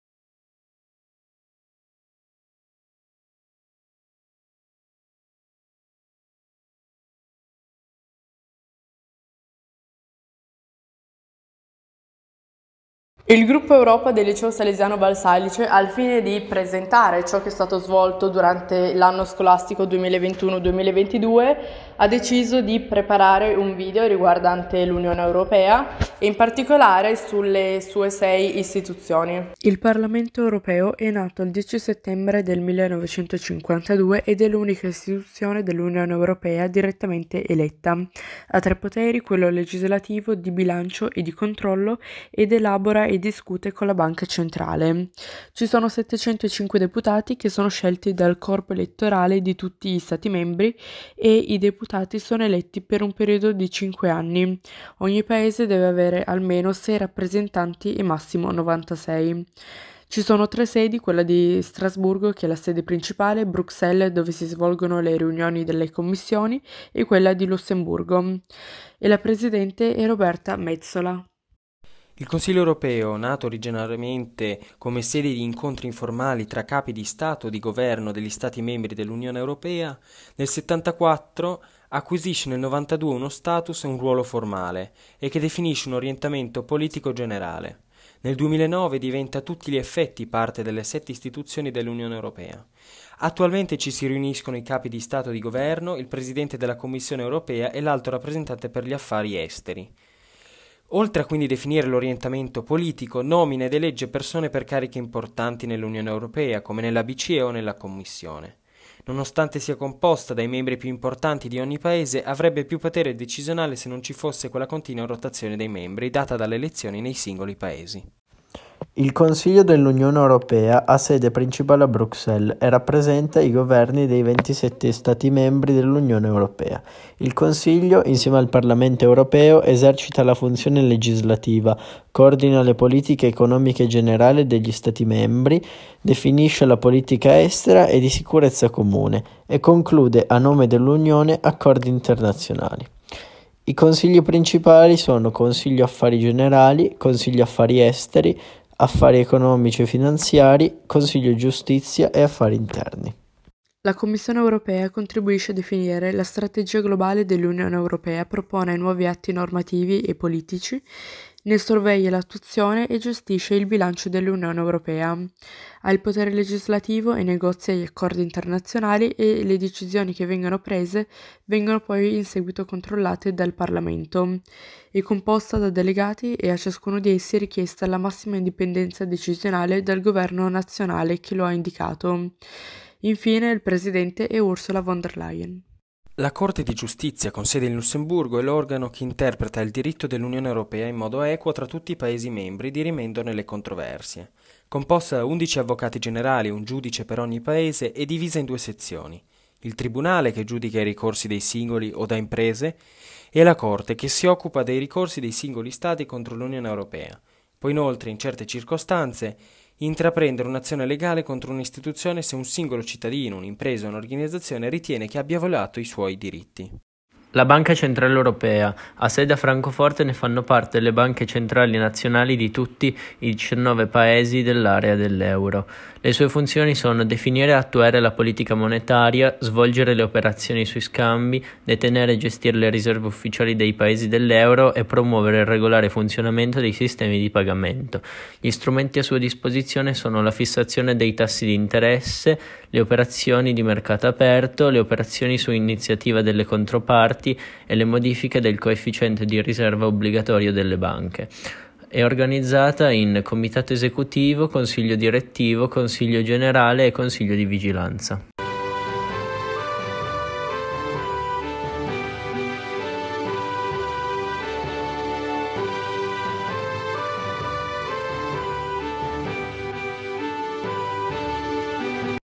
Le istituzioni europee
Liceo Valsalice - Torino, prof.ssa Silvia Scaranari: Le istituzioni europee. Lavoro presentato nell'ambito del progetto Ambasciatori del Consiglio regionale 2021/22 (Consulta regionale europea).